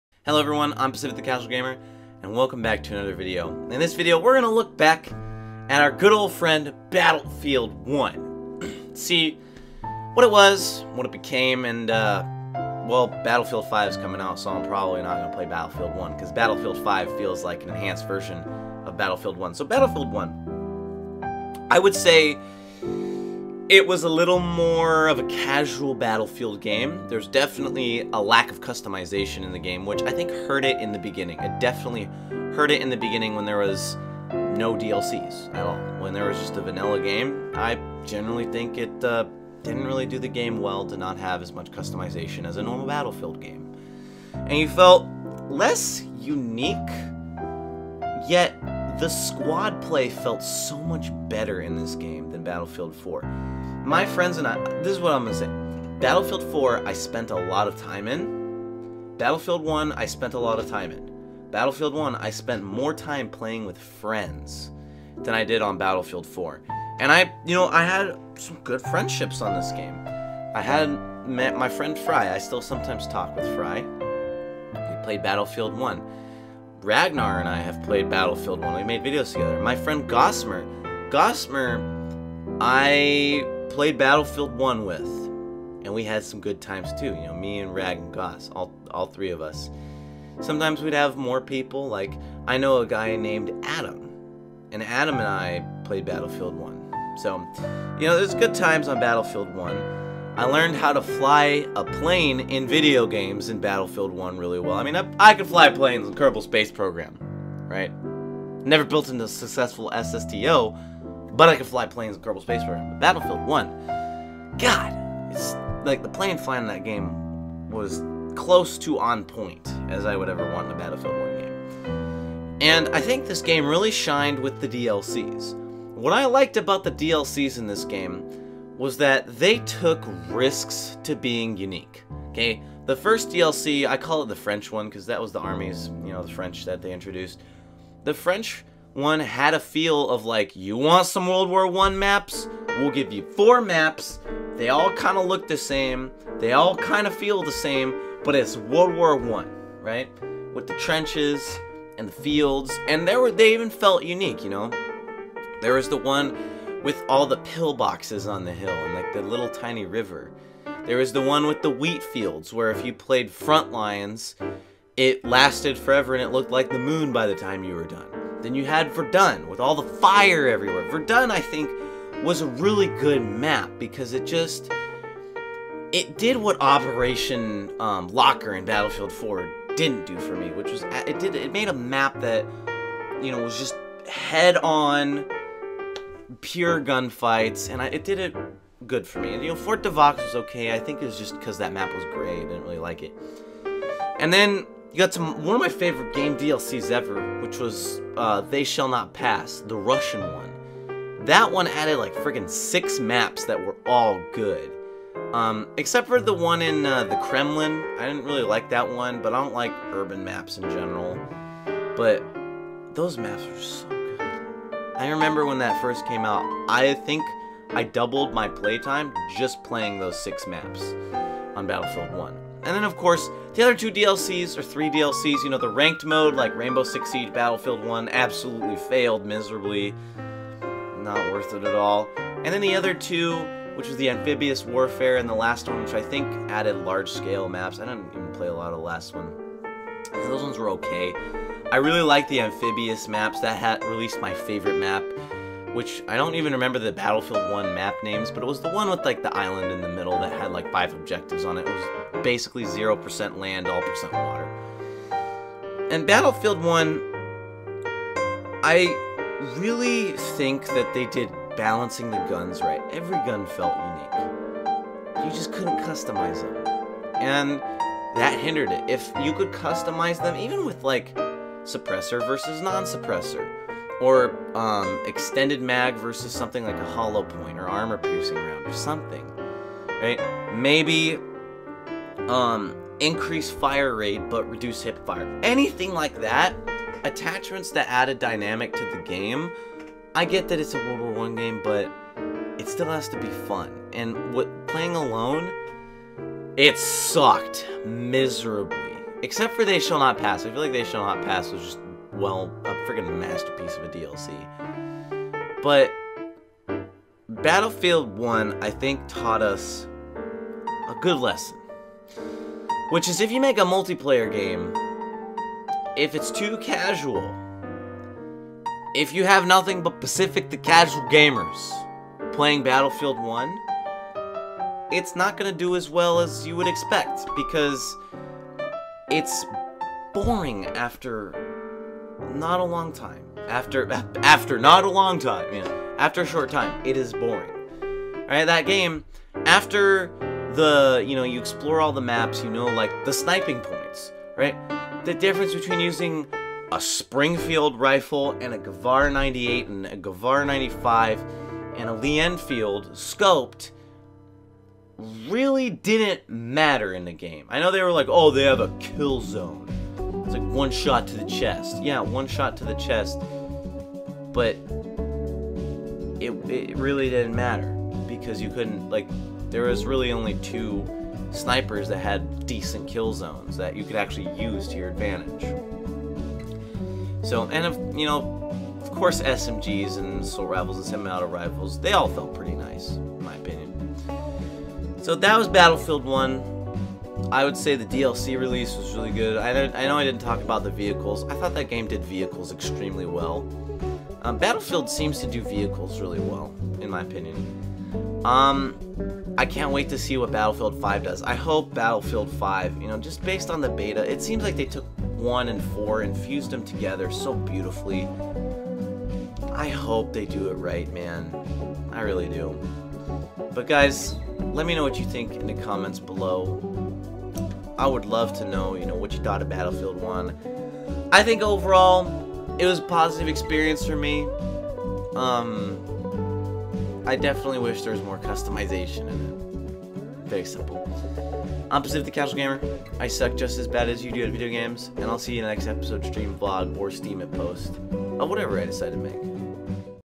A Look Back At Battlefield 1
Now that Battlefield V is basically out in it's 1.0 version, I think it's time to talk about Battlefield 1.

As a solo game, Battlefield 1 sucked.  However, I think it really shined as a team game.  Most of my fun memories with friends across the Battlefield franchise has been on Battlefield 1.  We've done challenges, messed around with vehicles, and even got so good at flying the bomber that we would hunt fighter planes down.  I leave this game with positive memories, and I hope the same goes for Battlefield V.